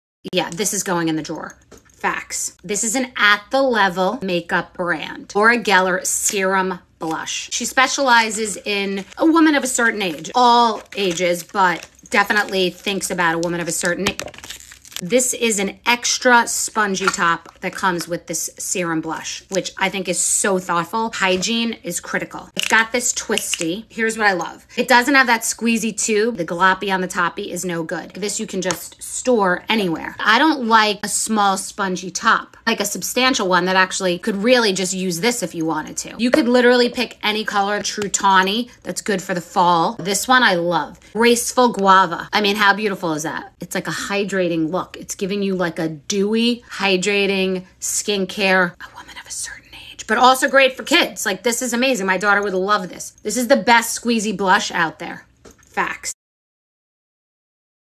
Bethenny Frankel loves our Serum Blush Cheek Tint | Laura Geller Beauty
If Bethenny Frankel says our Serum Blush Cheek Tint is the winner of the Best Squeezy Blush Award, then it must be true!🏆

“I’ve used this brand longer than I can remember and absolutely love how thoughtful they are! This Serum Blush Cheek Tint feels amazing and gives you that dewy, hydrating skincare feel with gorgeous color. Easy to clean, store, and better yet apply! It’s THE BEST, and I love it.”- Bethenny 😍

Bethenny loves the shades Beautiful Blossom, True Tawny, and Graceful Guava!